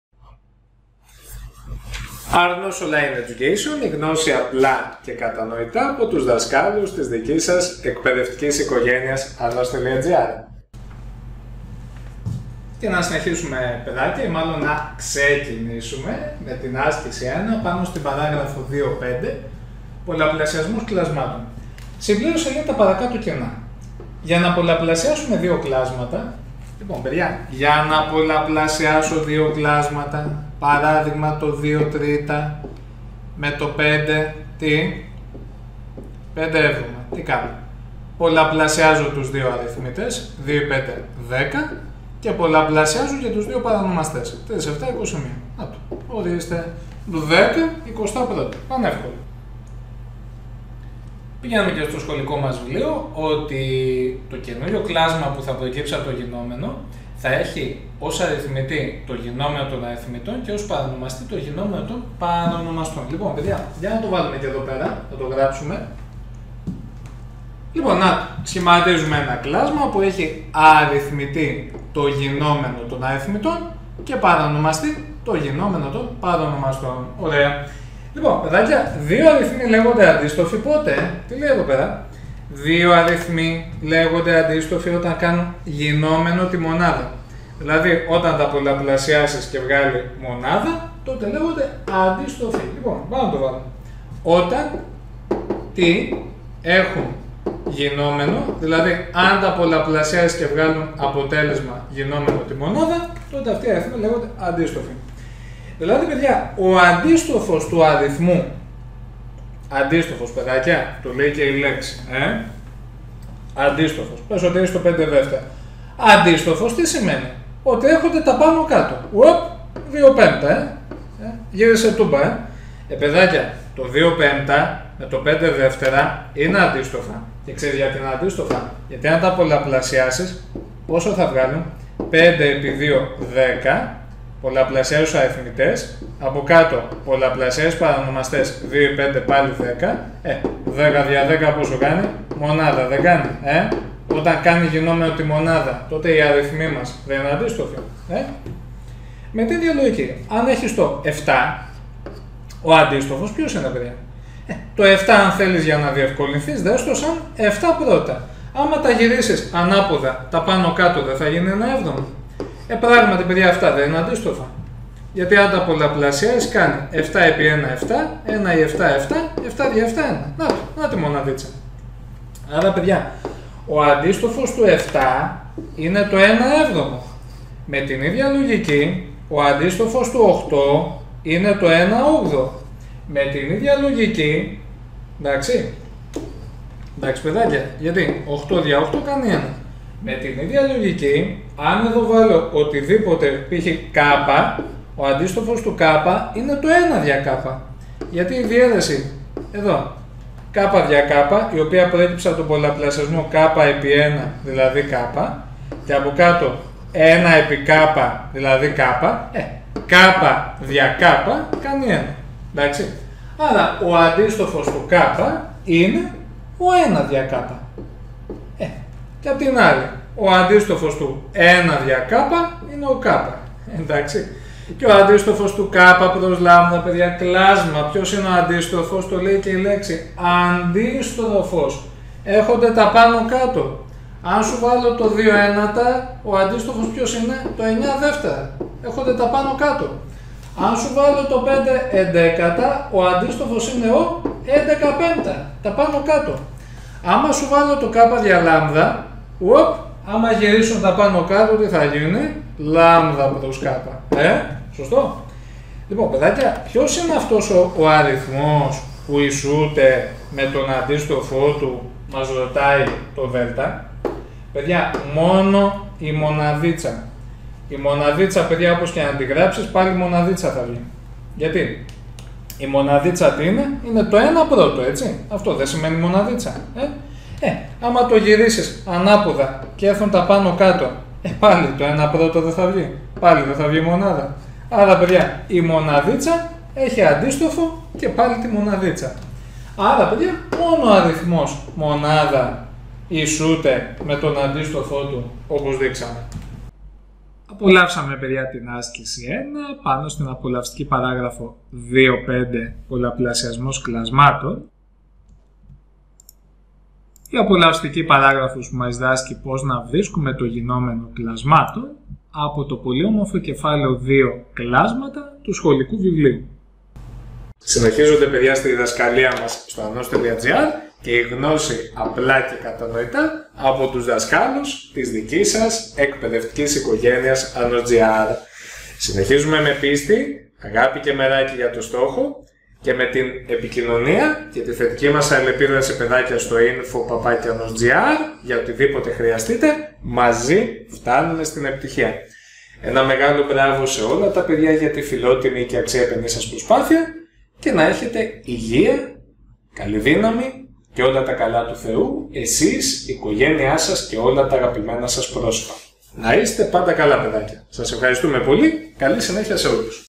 Μαθηματικά Α' Γυμνασίου Λυσάρι - 2.5: Πολλαπλασιασμός κλασμάτων - 1η Άσκηση Πρόβλημα
Μαθηματικά Α' Γυμνασίου
Κεφάλαιο 2ο: Τα κλάσματα
Ενότητα 2.5: Πολλαπλασιασμός κλασμάτων